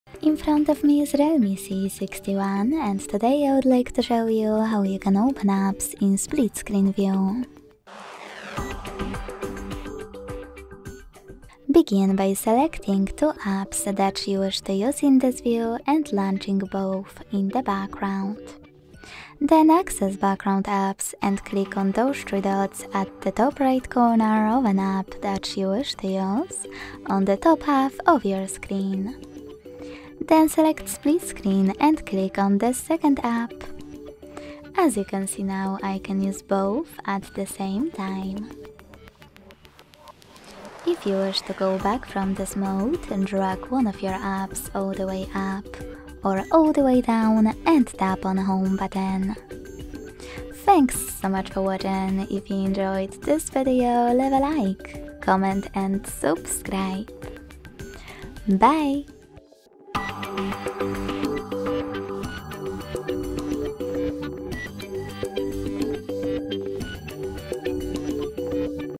REALME C61 - How to Enter Split Screen | Enable Multi-Window Mode
If you're interested in using multiple apps at once on your Realme C61, the split-screen feature is a great tool. This mode allows you to multitask effectively by viewing and interacting with two apps simultaneously. Follow along to see how you can enable and use split screen on your device to boost your productivity.

How to enable split screen in Realme C61?
How to activate multi-window mode in Realme C61 Smartphone?
How to use split screen feature in Realme C61 Phone?